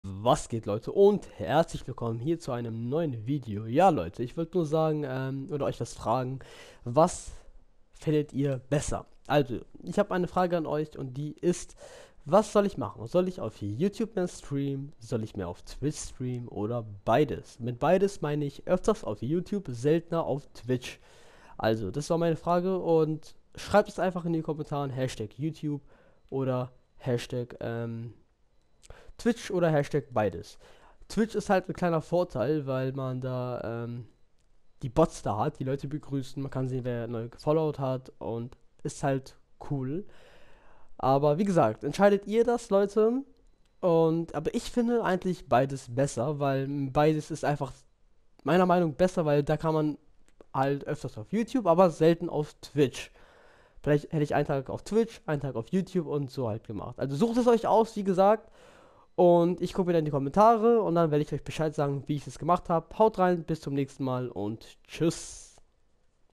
WORAUF SOLL ICH STREAMEN???
Hallo leute ihr habt richtig gehört Soll ich Auf Twitch Stream YouTube Oder Mehr Auf YouTube als Twitch wir ihr wollt.

Und zu Cobra

Zu Criimez